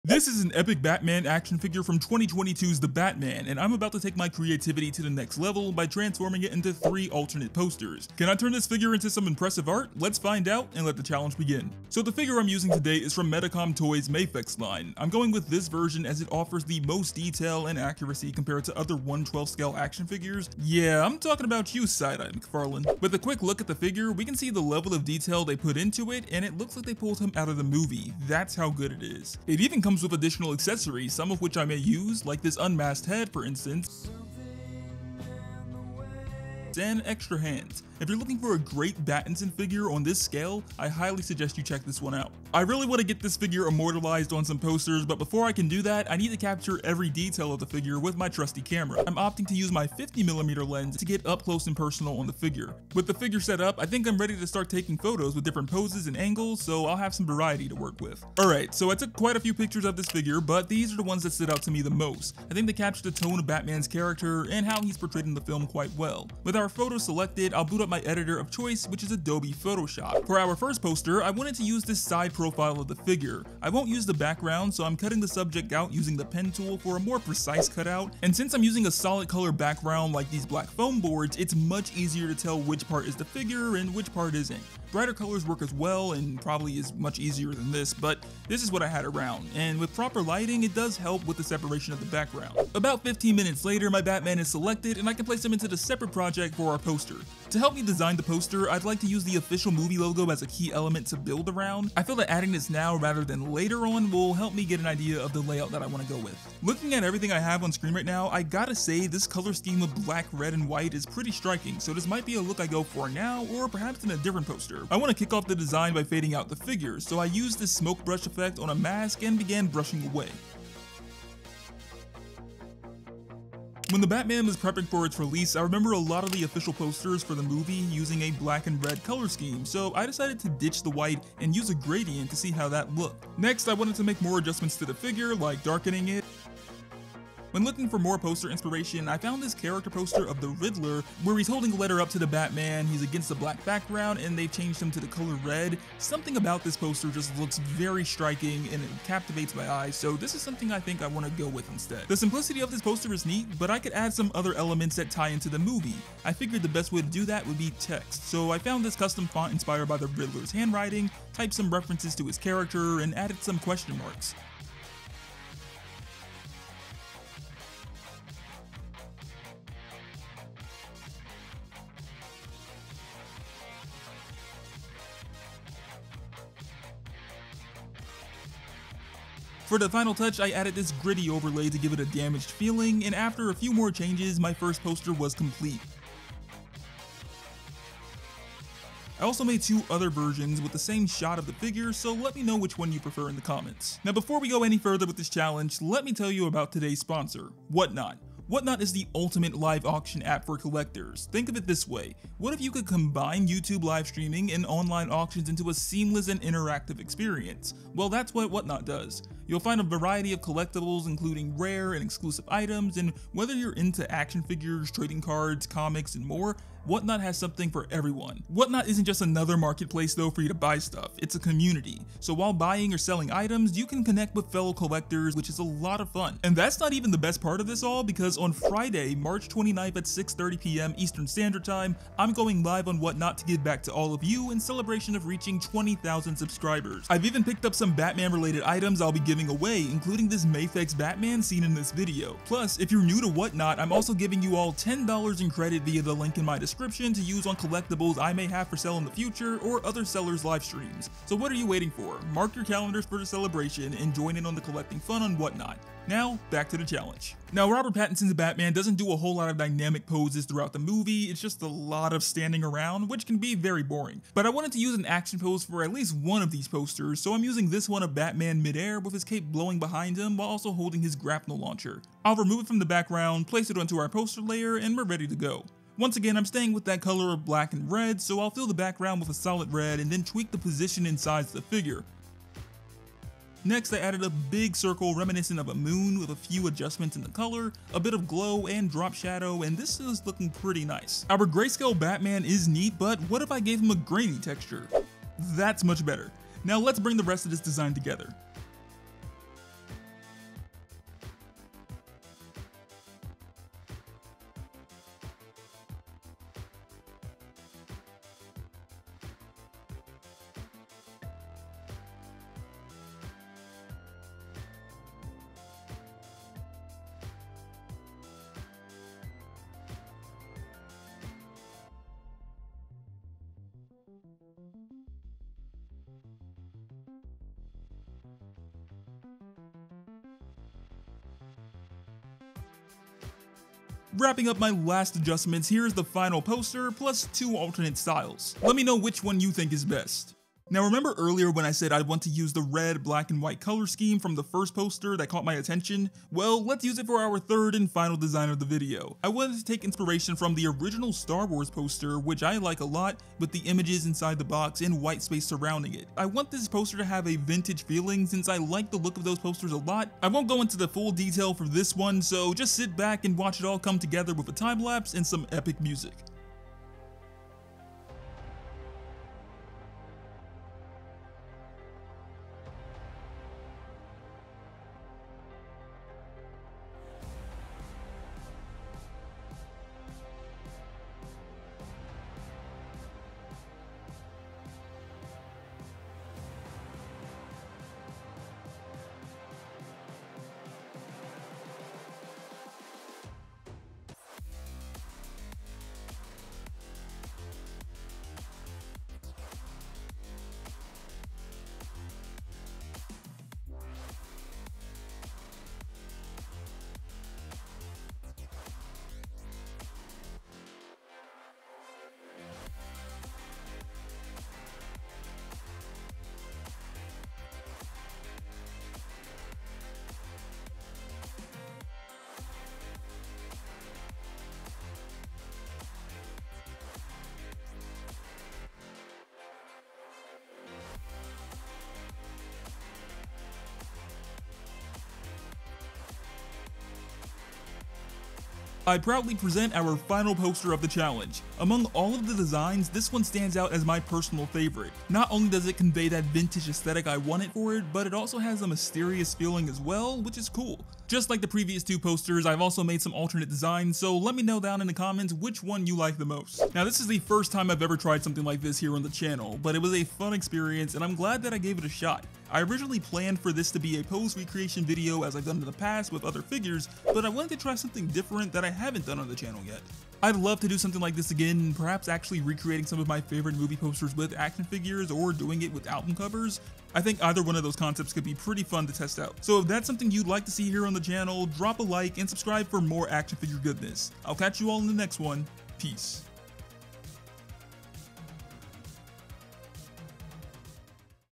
I Made Movie Posters with an Action Figure!
I challenged myself to create three alternative movie posters with the Mafex 188 Batman action figure with Adobe Photoshop!

0:00 Intro
1:09 Poster 1
4:18 Poster 2
5:41 Poster 3